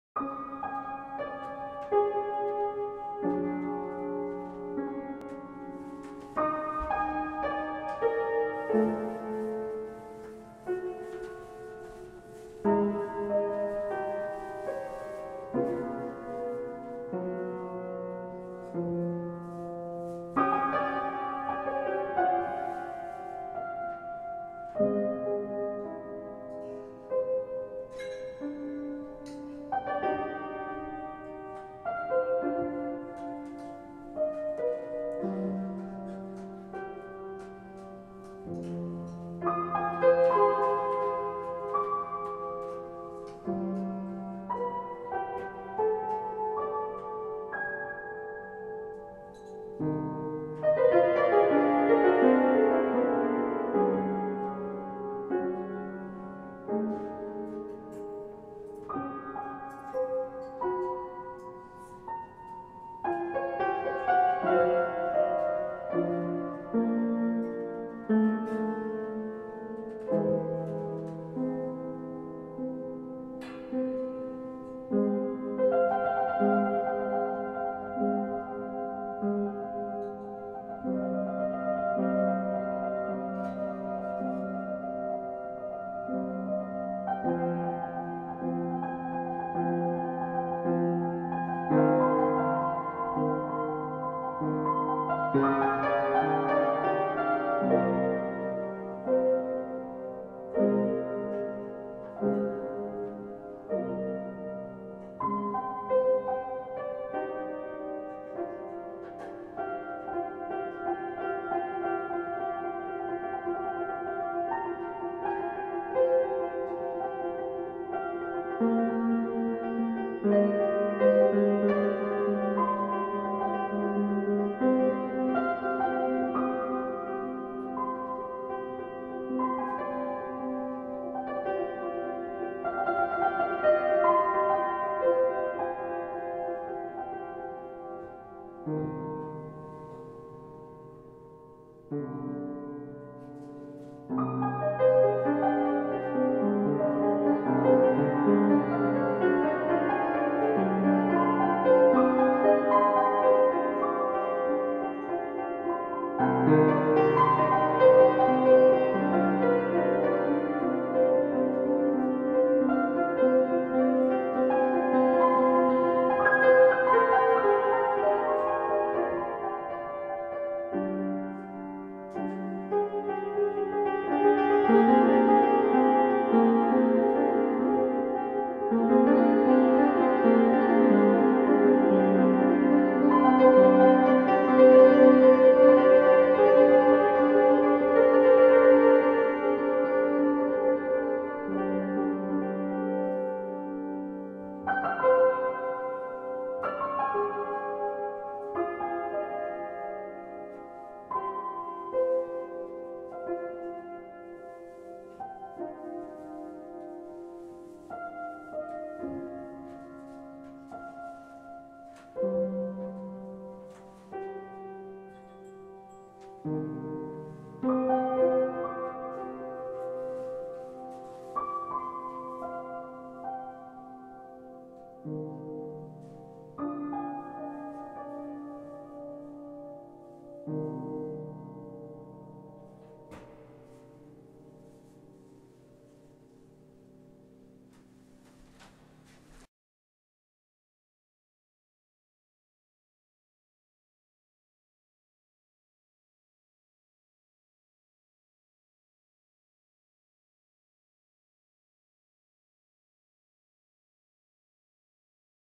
Minimal royalty free music piano for video background
Download it and use this subtle and serene piano music track royalty free for background music for videos or films. Just give credit and don't use for commercial purposes please.